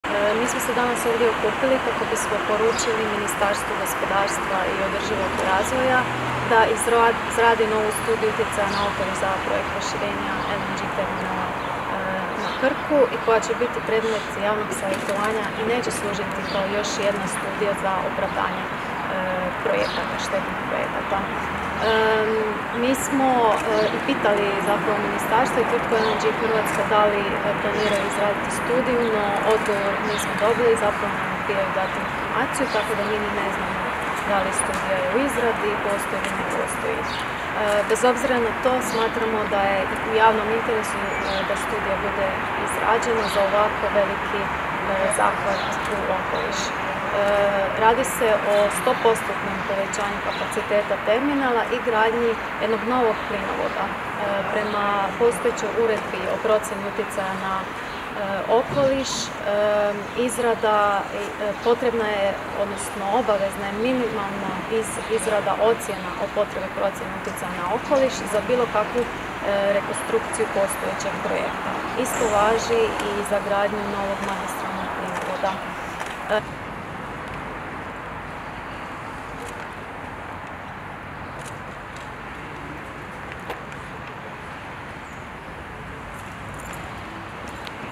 Zelena akcija traži novu okolišnu studiju za širenje LNG terminala
Zagreb, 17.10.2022.- Zelena akcija zatražila je u ponedjeljak od Ministarstva gospodarstva i održivog razvoja da izradi novu studiju utjecaja na okoliš za projekt proširenja krčkog LNG terminala i pošalje je u javno savjetovanje. Govori potpredsjednica Zelene akcije i voditeljica kampanje za ukidanje plina Marija Mileta nakon performansa održanog jutros ispred zgrade Ministarstva gospodarstva i održivog razvoja.
video HINA/ Tomislav PAVLEK/ ua